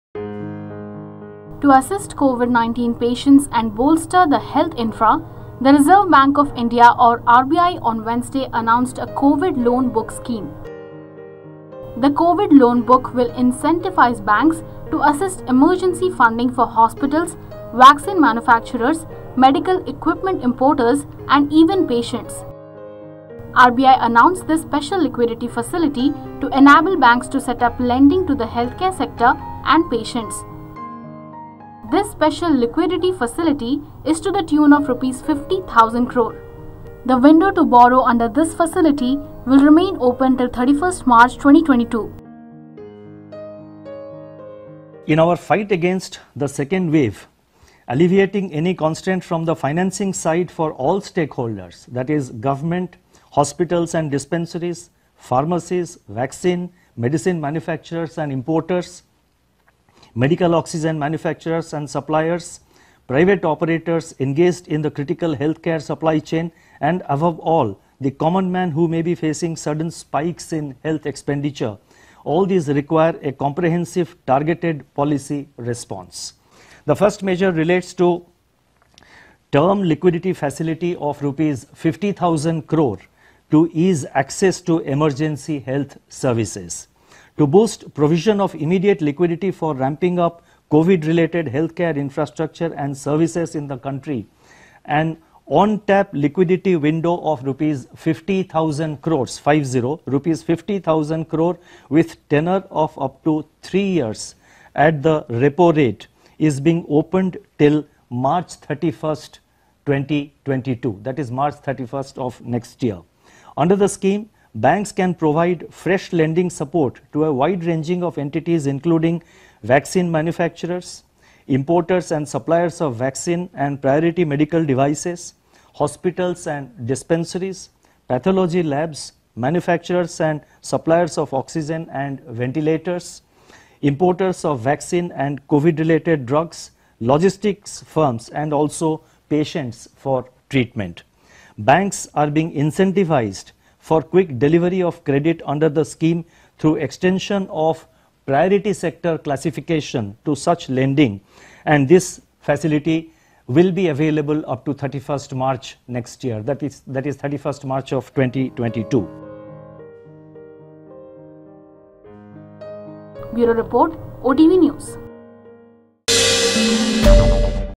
RBI Announces Covid Loan For Patients & Health Sector | OTV News
To assist COVID19 patients & bolster the health infra, the Reserve Bank of India (RBI) on Wednesday announced a ‘Covid Loan Book’ scheme. The Covid Loan Book will incentivise banks to assist emergency funding for hospitals, vaccine manufacturers, medical equipment importers and even patients.